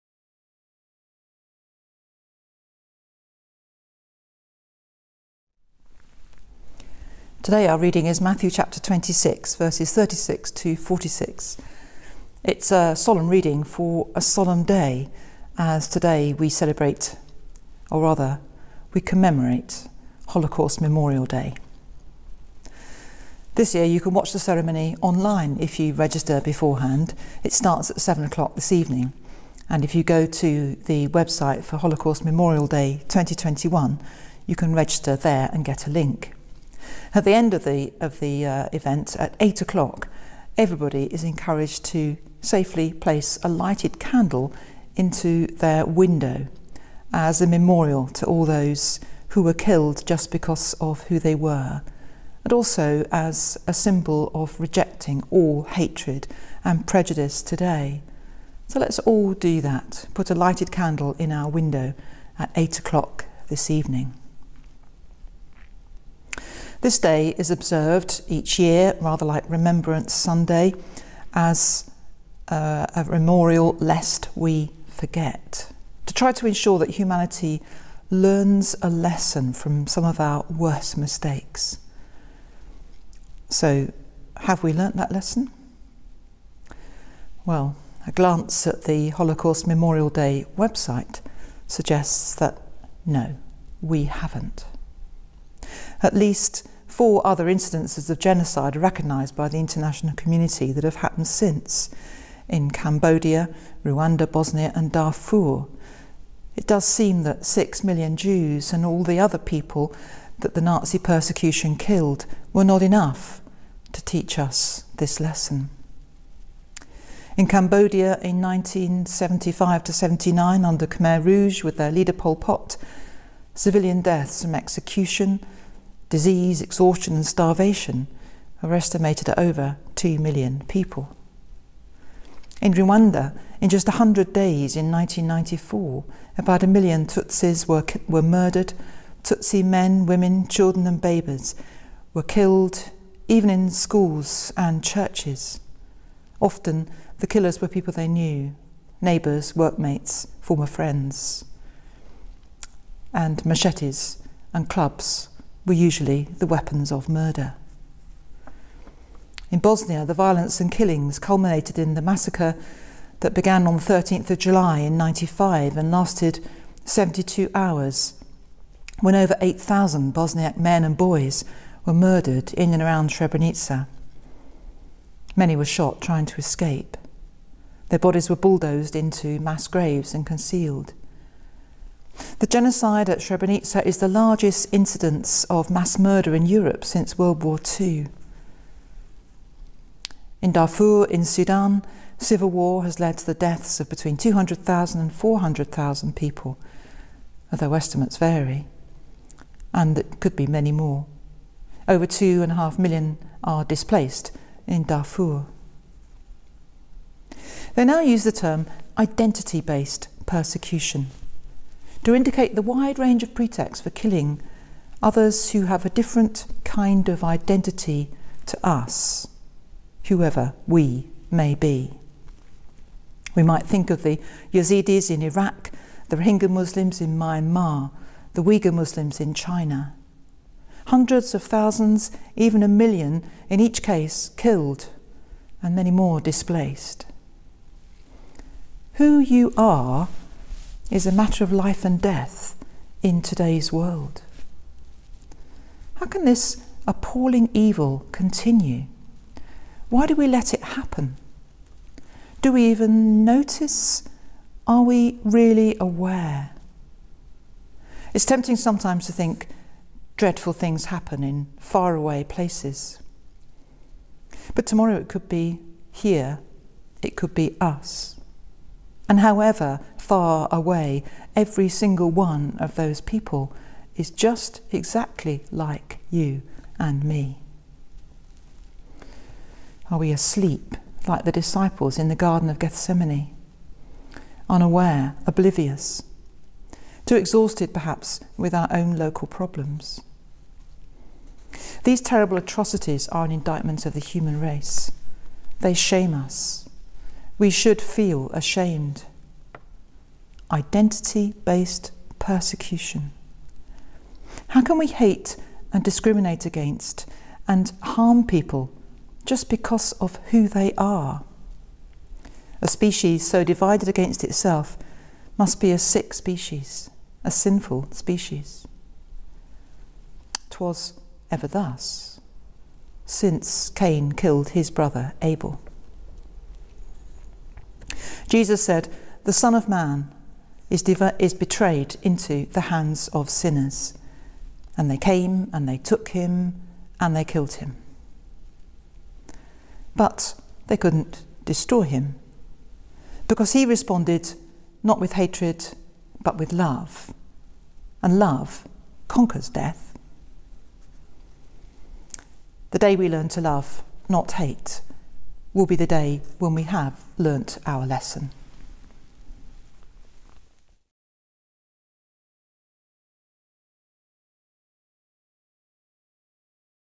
Thought for the Week - 27th January
Canon Jan van de Lely presents her Thought for the Week on Matthew 26.36-46. Today we commemorate Holocaust Memorial Day. Click the following link to go to the event page mentioned by Canon Jan